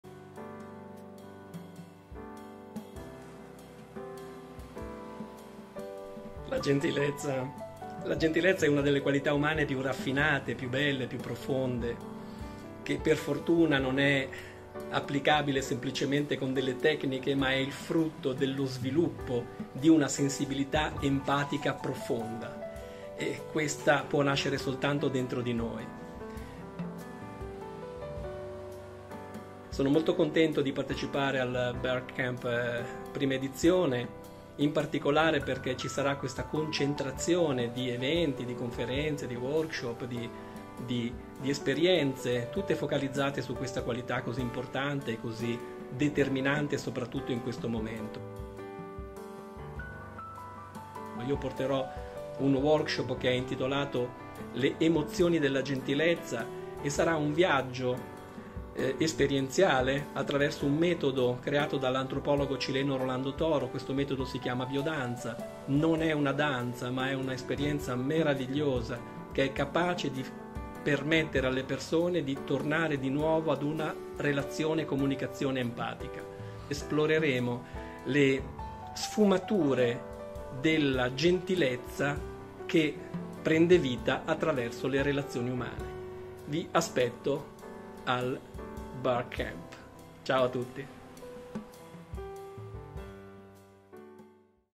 Ugo Rizzo al primo Barcamp sulla Gentilezza
Ugo Rizzo - Giornalista, Facilitatore relazionale, Mental Coach sarà uno dei relatori al primo barcamp sulla Gentilezza in programma a Ivrea, dal 17 al 18 giugno 2022.

L'evento, organizzato da CIAC insieme alla Fondazione Committo Impresa Sociale e in collaborazione con il progetto nazionale Costruiamo Gentilezza, si terrà presso la Scuola Alberghiera CIAC "F. Prat" di Ivrea.

Partecipa anche tu al primo BarCamp sulla Gentilezza in Italia, a Ivrea Capitale del Libro 2022!

👉Vuoi saperne di più dei nostri relatori?

👉Vuoi partecipare anche tu all'evento?

Ti aspettiamo!